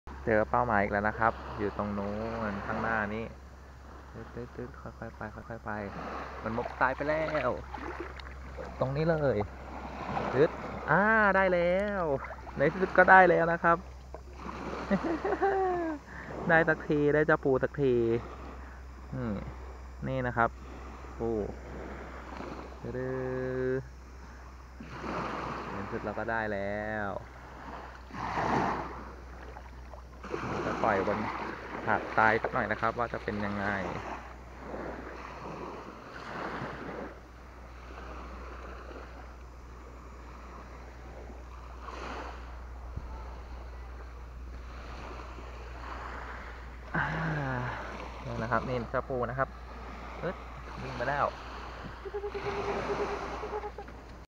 ชายหาดหัวหิน จับปูเล่นตอนเช้า action camera EKEN H9R 1080P 60FPS
ทดลองกล้อง action camera EKEN H9R 1080P 60FPS ไม่ใส่เคส คลิปนี้อัดที่ชายหาดหัวหินวันที่ 21/05/2018